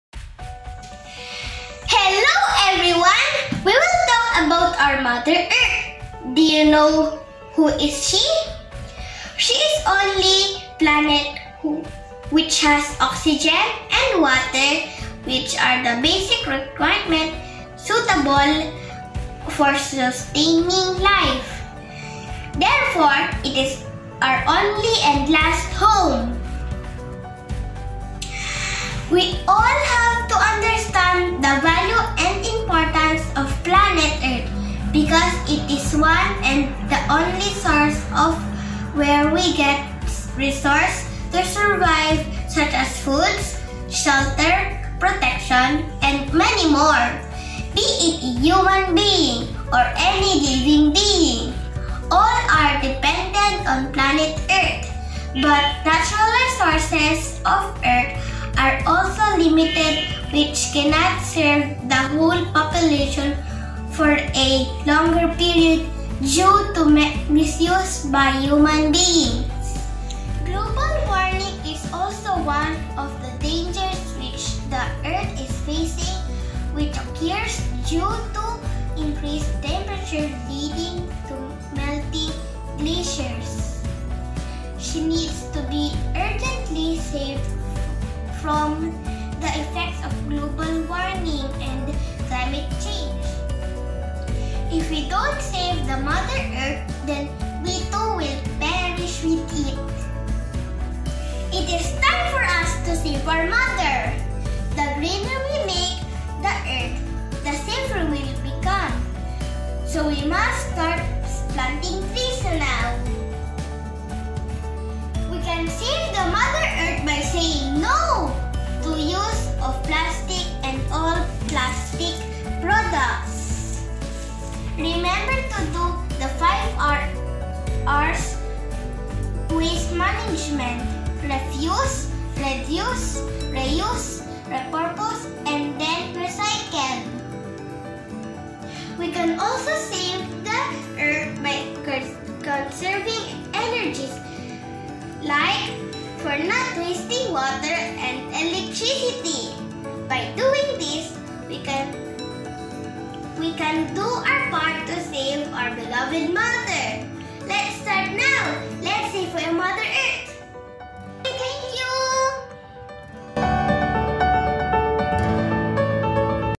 Save the Earth Awareness (Science 2 Project)
It is time to save the mother earth for the next generations!